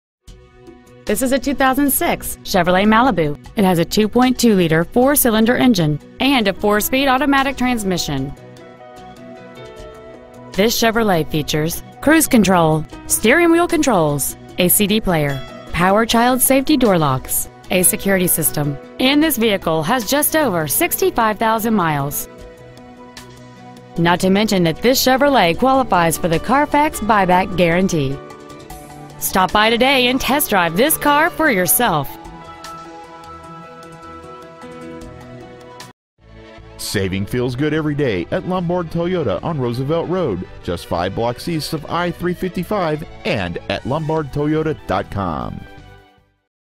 Pre-Owned 2006 Chevrolet Malibu Lombard IL
Year: 2006
 Make: Chevrolet
 Model: Malibu
 Engine: Gas 4-Cyl 2.2L/134
 Trans.: Automatic
 Exterior: Sport Red Metallic
 Miles: 65,530
 Interior: Cashmere

 725 W. Roosevelt Road

 Preowned 2006 Chevrolet Malibu Lombard IL